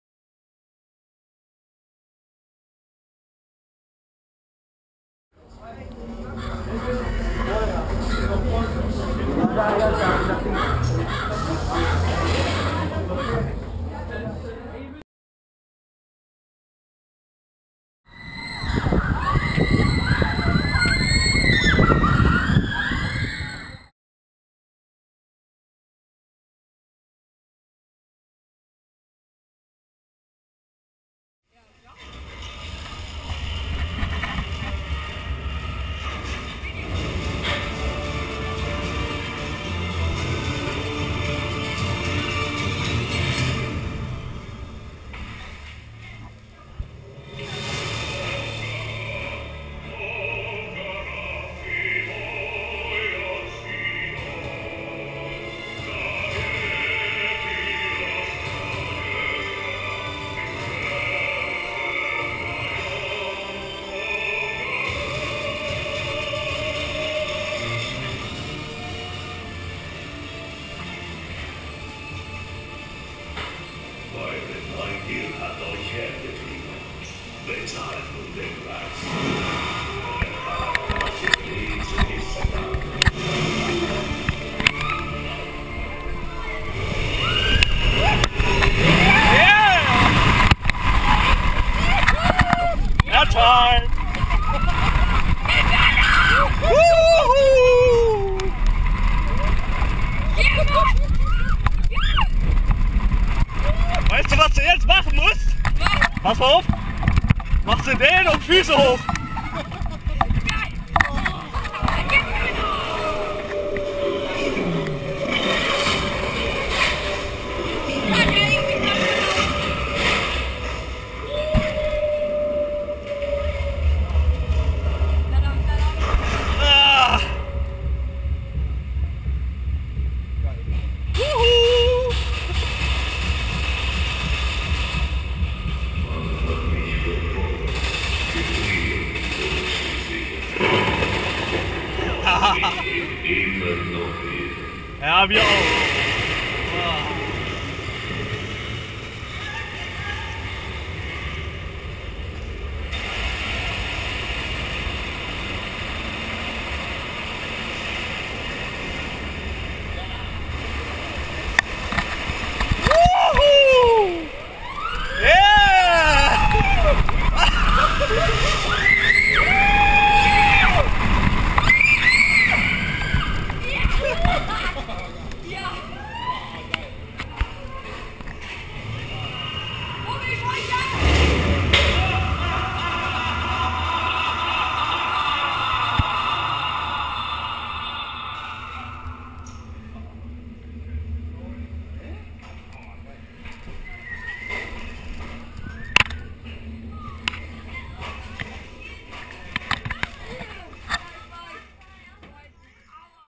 Fluch von Novgorod onride HD -Weitwinkel-
Onride-Video from Fluch von Novgorod at Hansa Park, Germany.
Filmed with 1280x960p, 30fps.

Fluch von Novgorod. Ein Launch-Coaster von Gerstlauer. 1,4 Sekunden von 20 auf 100km/h, Heartlineroll, vertikaler Lift, 97° Drop.
Die geniale Gestaltung und Spezialeffekte, wie z.B. die Animatronics runden diesen Coaster ab. Für mich die Perfekte Mischung aus Desert Race und Blue Fire.